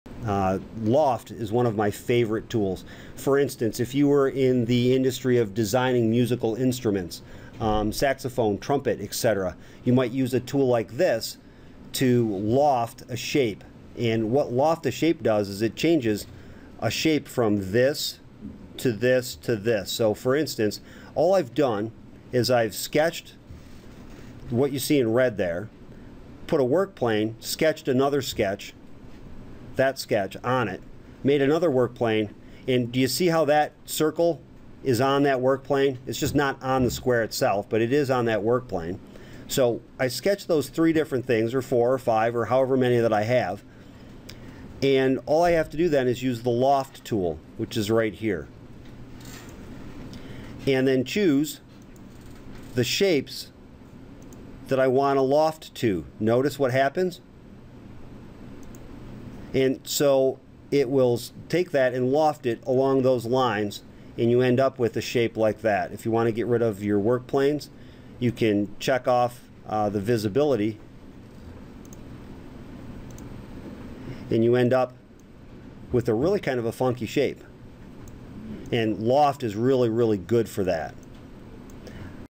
Inventor: Loft
What is a loft used for, and how do you do one in Inventor?